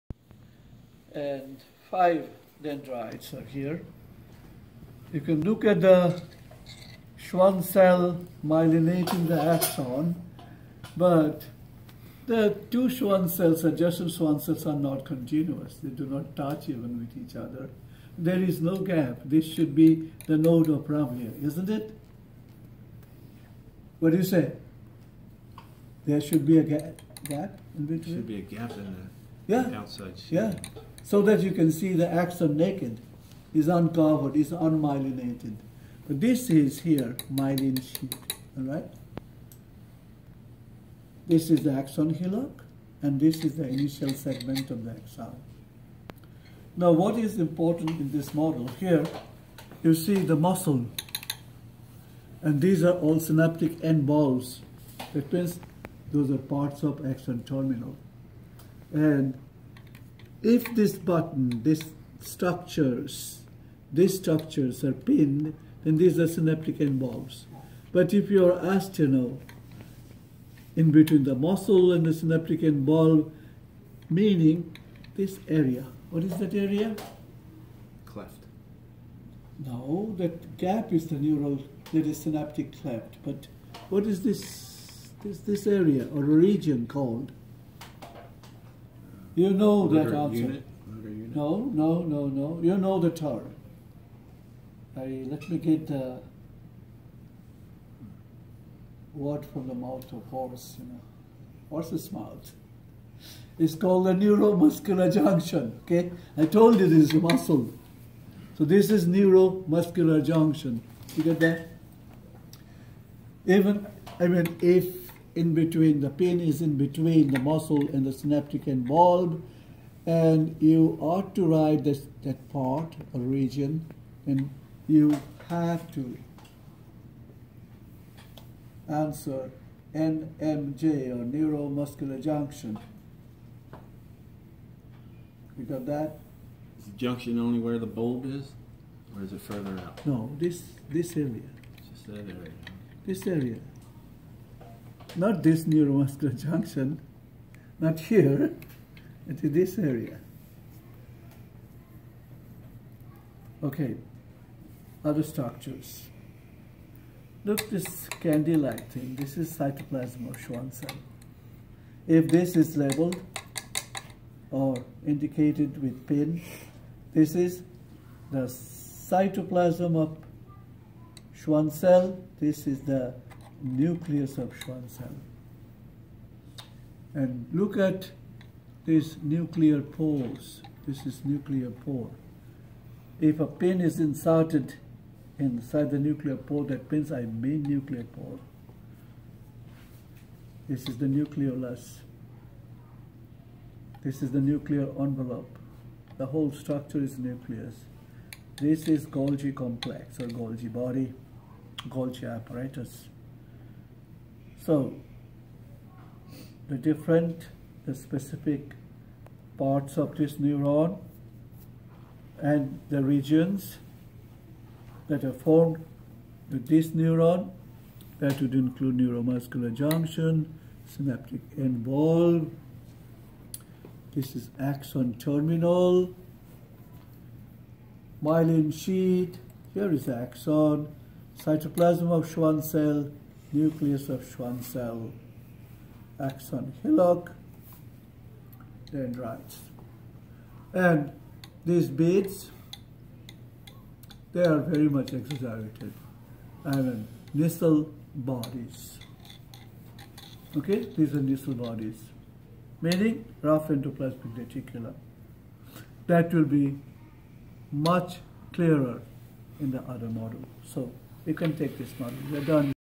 Multipolar Neuron Model with NMJ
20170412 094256
Dendrites, axon hillock, initial segment of axon, axon terminal, synaptic end bulbs at neuromuscular junction, Schwann cell, nuclear pore, nuclear envelope, nucleolus